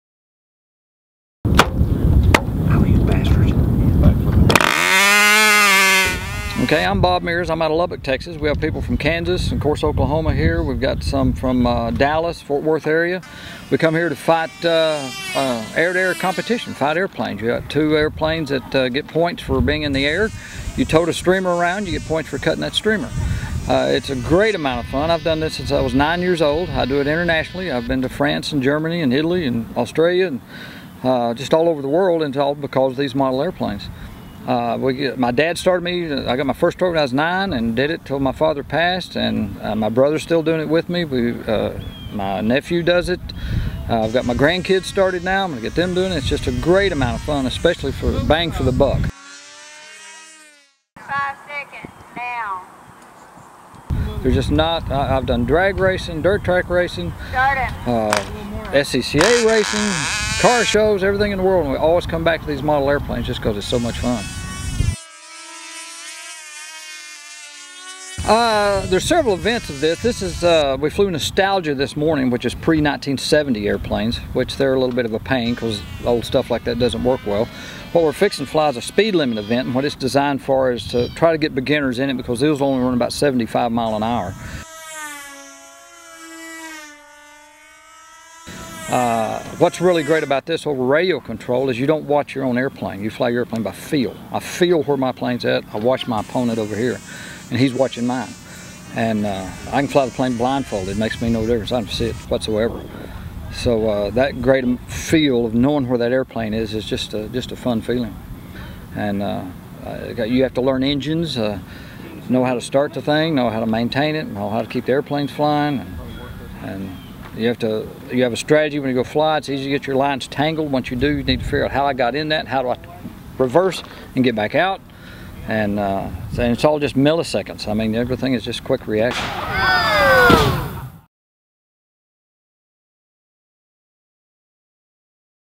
Control line combat model plane competition.
A model aviation competition put on by TORKS (The Oklahoma Radio Kontrol Society) at Tom Jones Memorial R/C Field in Oklahoma City, Okla., Saturday, Sept. 19, 2020.